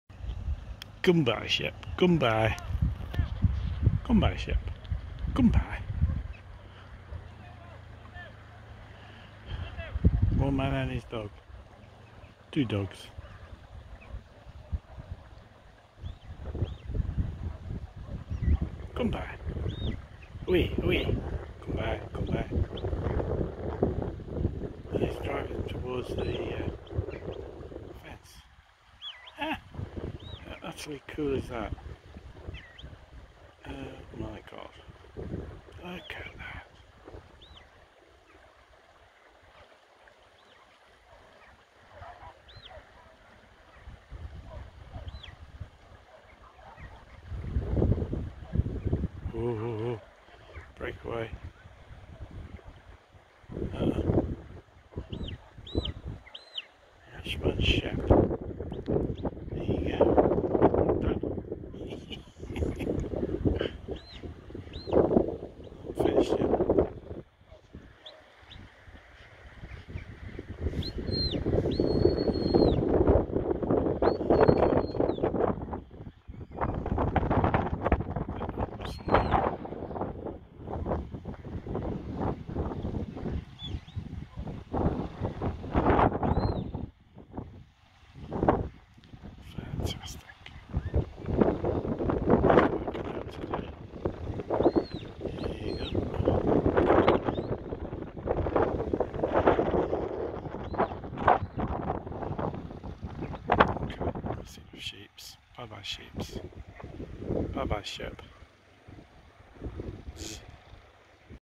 Border Collies Herding Sheep In The Border Country Between Scotland And England On The Pennine Way
Experience the mesmerizing skill of Wolf and Meg, two expert Border Collies, as they navigate a flock of sheep through the breathtaking open countryside of the Cheviot Hills. This captivating journey unfolds next to the legendary Pennine Way in Scotland, close to the serene village of Kirk Yetholm and the historic border with England. Watch as these intelligent dogs, under the watchful eye of their shepherd on a quad bike, demonstrate their herding prowess by moving the sheep from one pasture to another, showcasing the beauty and tradition of rural life in the Borders.

🐾 What's Inside:

An up-close look at Wolf and Meg's incredible sheep herding skills in the Cheviot Hills.
The shepherd's unique perspective, following the action on a quad bike.
Stunning visuals of the sheep moving seamlessly through the rich, open landscapes near the Pennine Way.
The tranquil beauty of the Borders, where Scotland meets England, near Kirk Yetholm.

If you're fascinated by the intelligence of Border Collies, the serene beauty of the Scottish-English countryside, or the simple elegance of traditional sheep farming, this video is a must-watch. 🛎️🌿